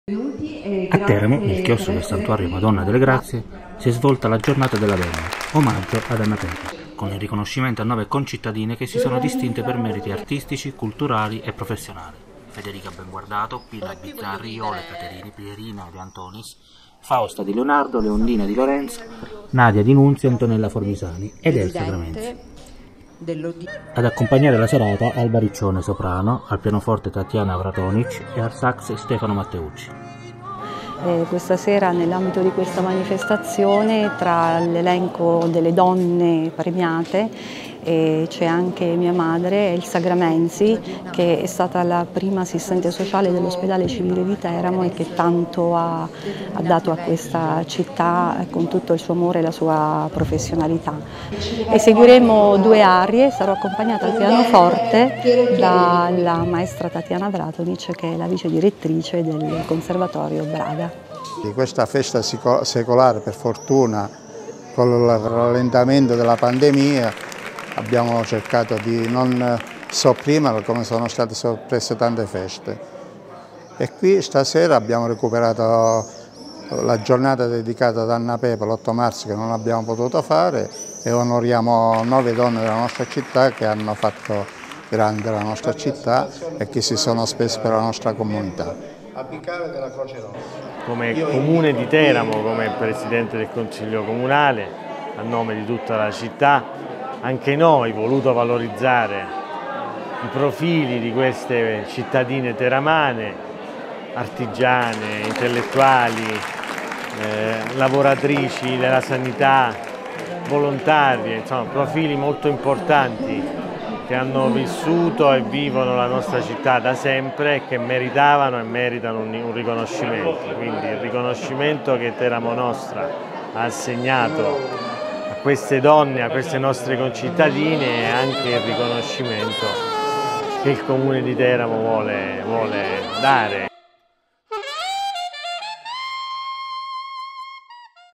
Teramo. Giornata della donna, omaggio ad Anna Pepe
L'Associazione Teramo Nostra ha organizzato una giornata della dona in omaggio ad Anna Pepe. 9 le donne teramane premiate nel chiostro della "Madonna delle Grazie